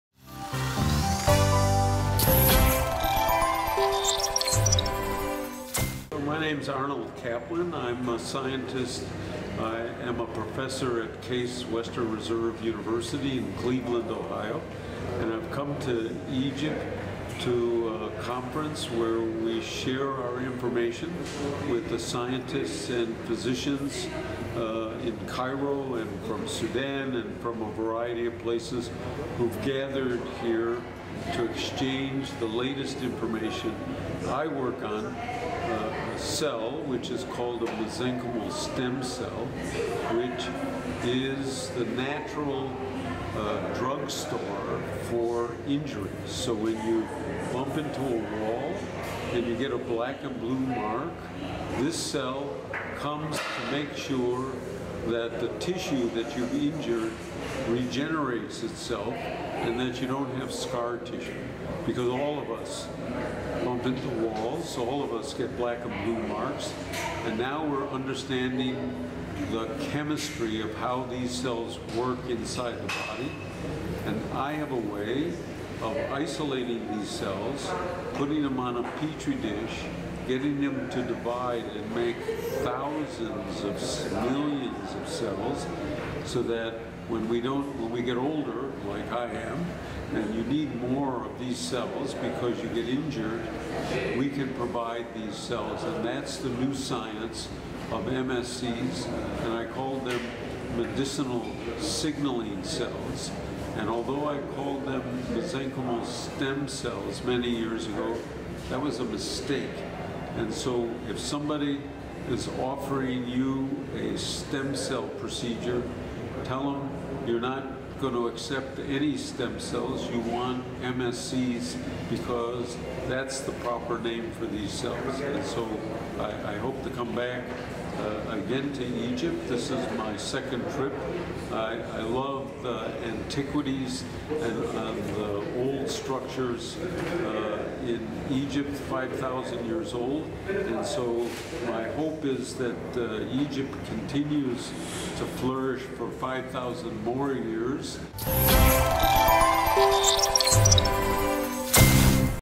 رائد الخلايا الجذعية يغير اسمها ويكشف عن تفاصيل استخدامها
د.أرنولد كابلن صاحب لقب "أبو الخلايا الجذعية" والعالم الأمريكي الذى أطلق الاسم عليها والذى غيره مؤخرا

شاهد ايضا